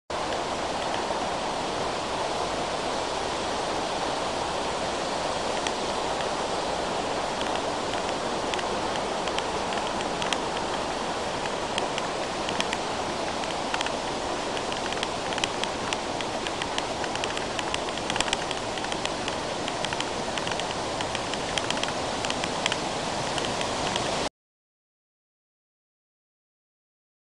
Walking up to Watson Falls 2
kept going up this trail through the beautiful rainforest in the Oregon Cascades in search of watson falls